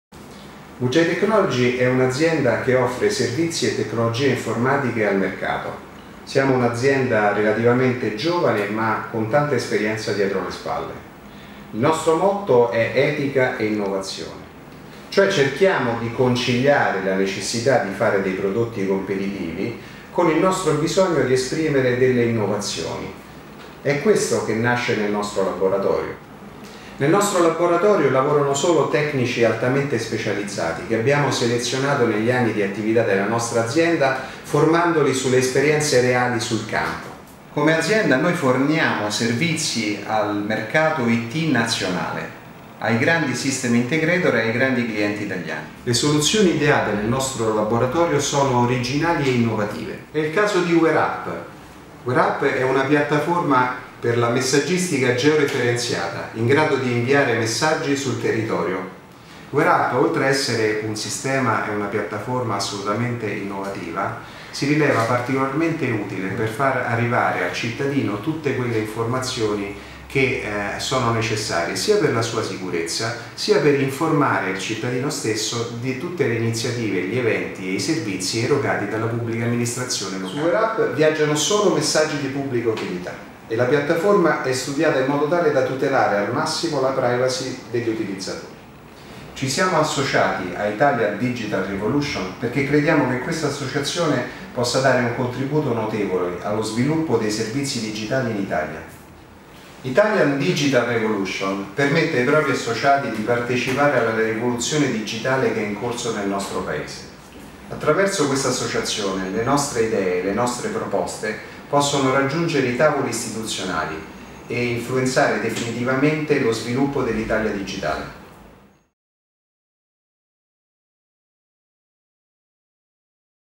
Mauro Santini, Direttore Marketing e Vendite VJTECNOLOGY
Il  Direttore Marketing e Vendite VJTECNOLOGY, Mauro Santini, presenta l'Azienda partner AIDR